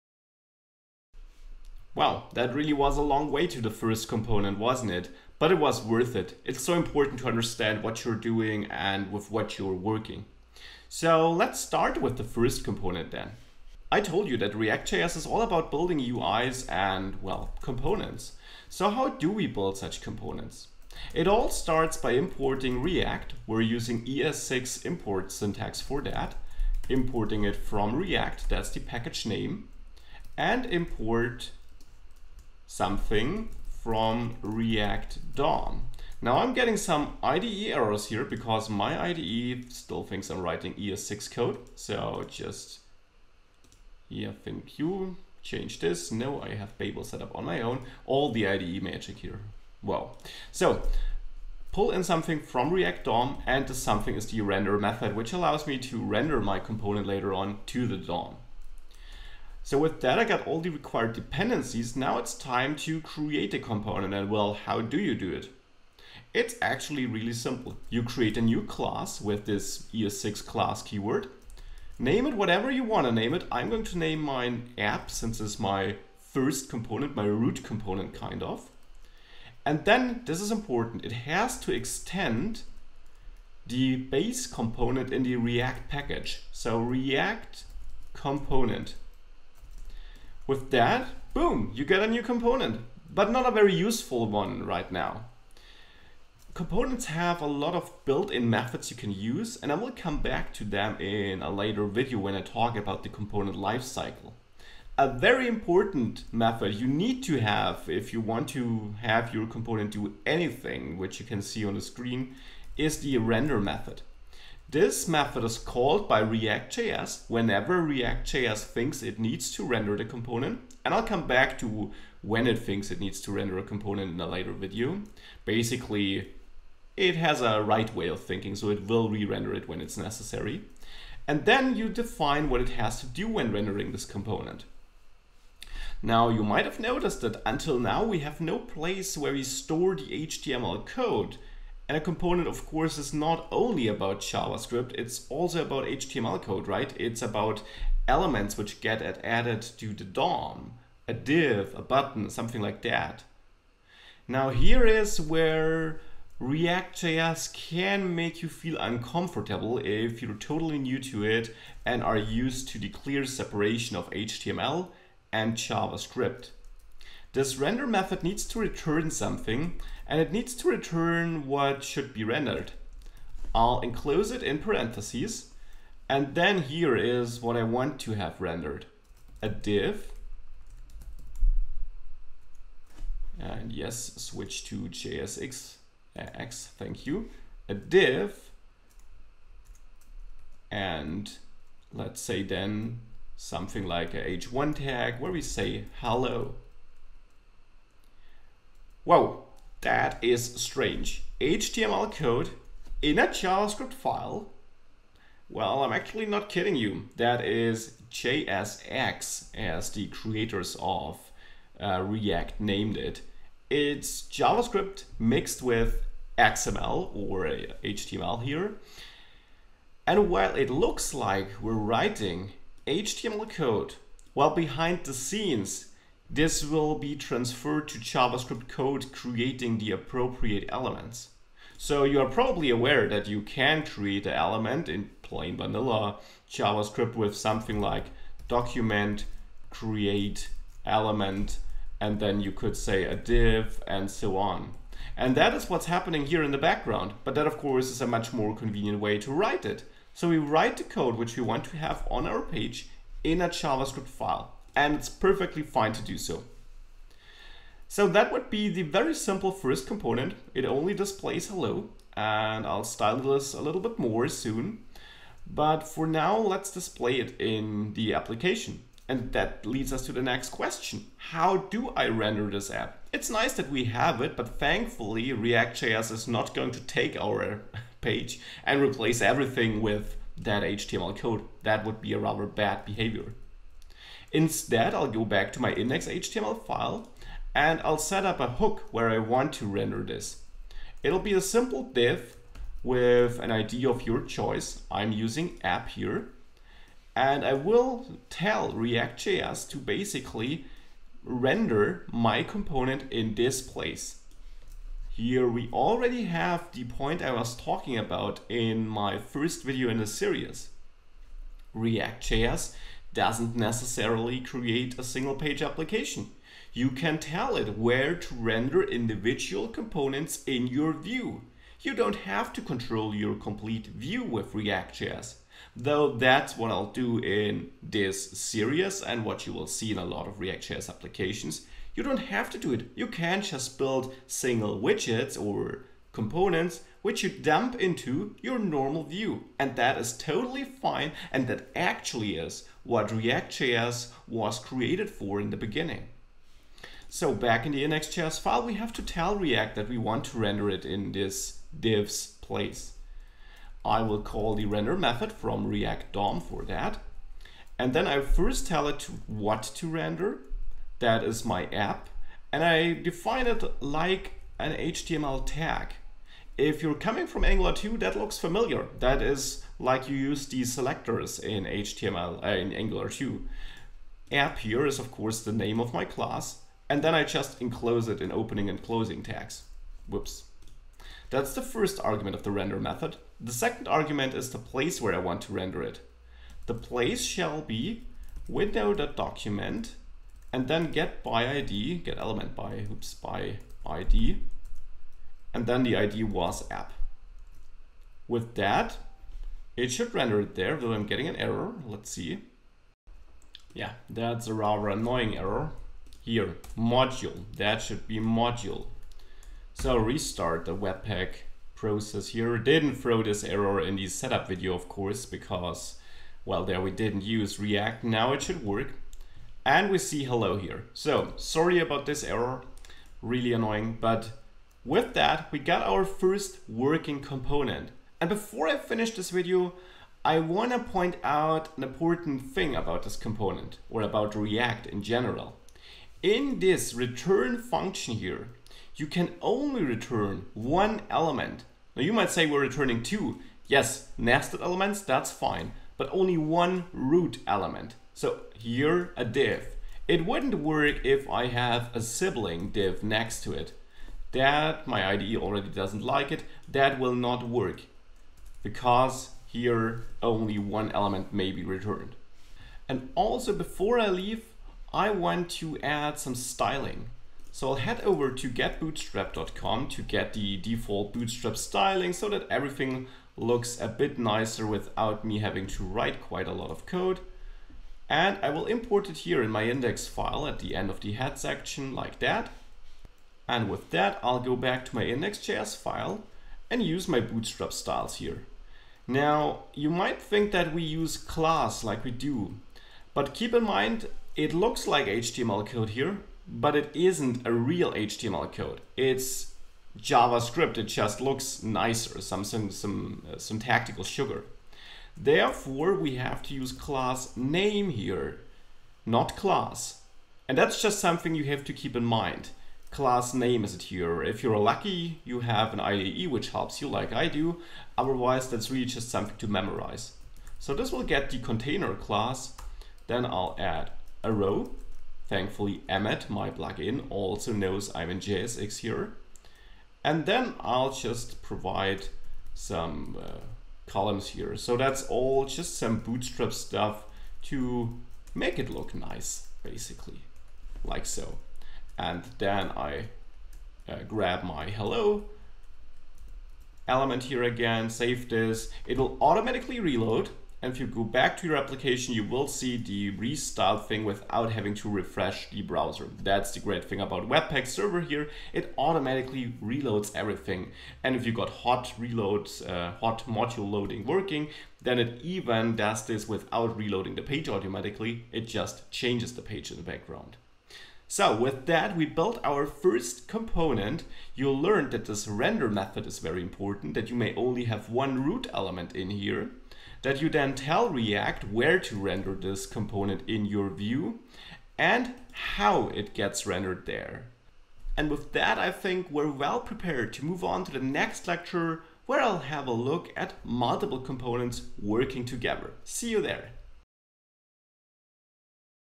ReactJS Basics - #3 First Component
Time to create the first ReactJS Component - that's what its all about, right? Also learn how to render it to the DOM.

It is Part of the ReactJS Basics Series.